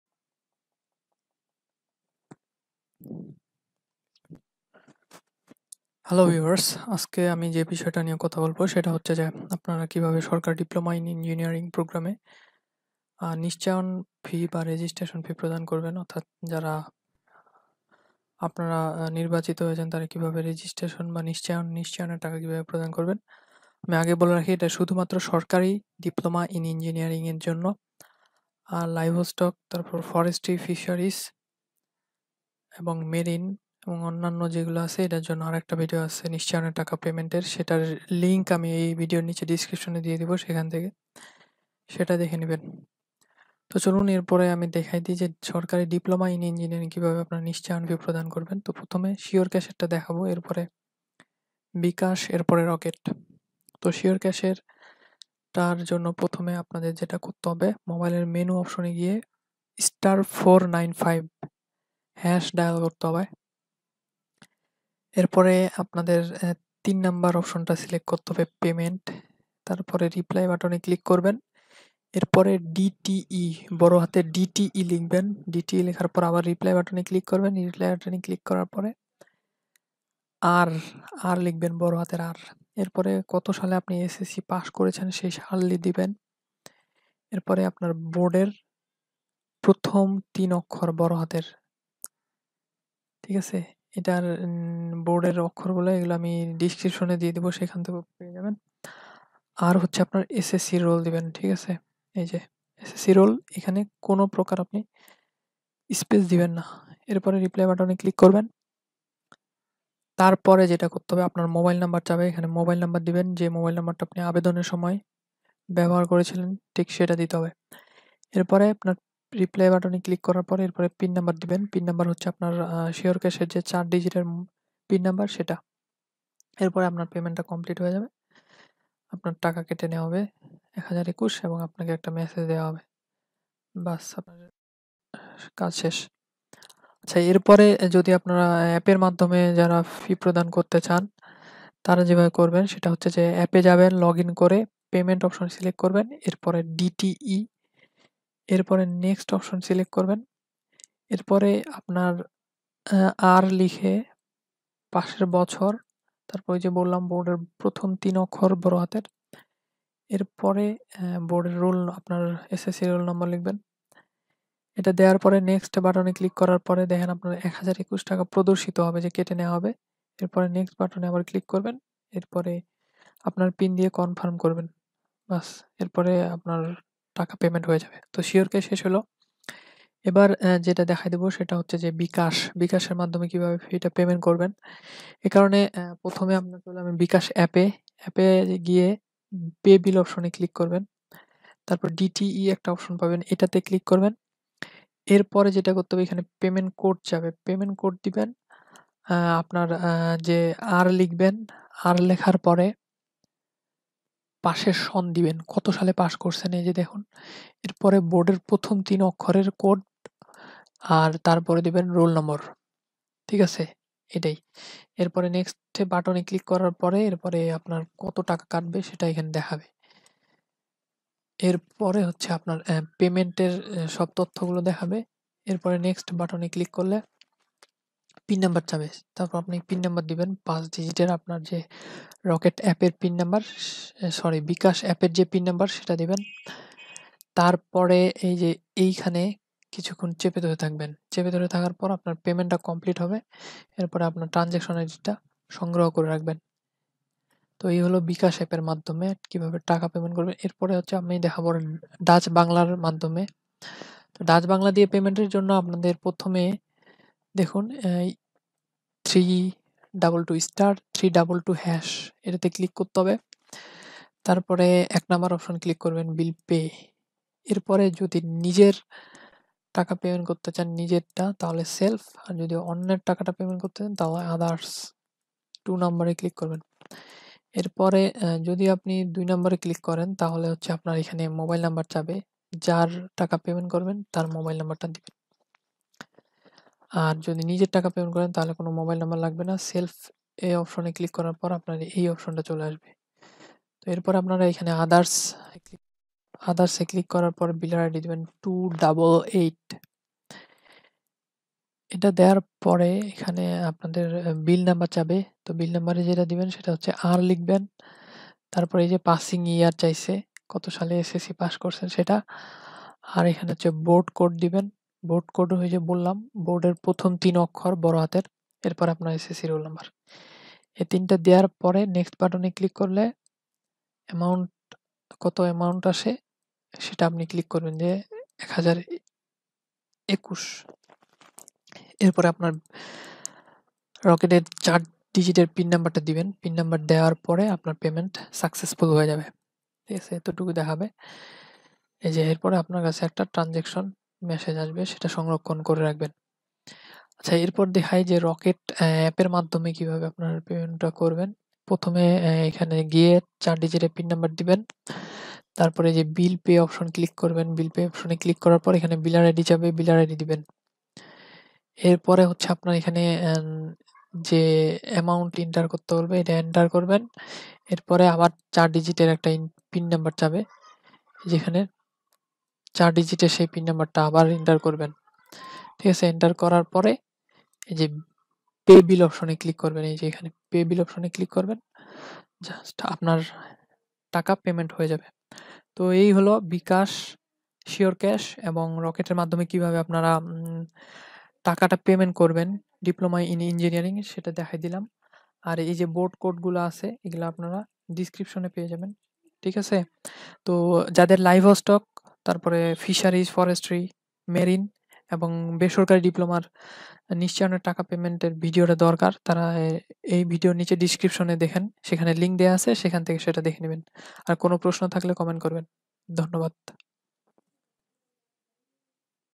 ডিপ্লোমা ভর্তি নিশ্চায়ন পদ্ধতি-২০১৯|Surecash|Rocket|bKash|Diploma Admission Registration process
ডিপ্লোমাইন ইঞ্জিনিয়ারিং ভর্তির জন্য নিশ্চায়ন টাকা প্রদান পদ্ধতি ,বিকাশ ,শিওর ক্যাশ ,রকেট এগুলোর মাধ্যমে টাকা প্রদান করা যাবে ||
 মেরিন /ফরেস্ট্রি/ লাইভস্টক/ ফিশারিজ /বেসরকারি ডিপ্লোমা ভর্তি নিশ্চায়ন পদ্ধতি-